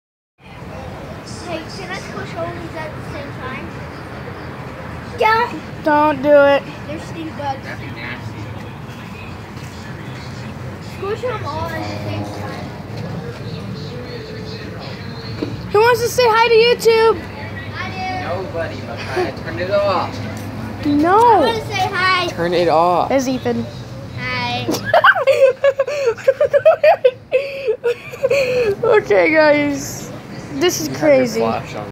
Junebug disaster
We were eating at Subway and we found a lot of June bugs and by a lot I mean a lot a lot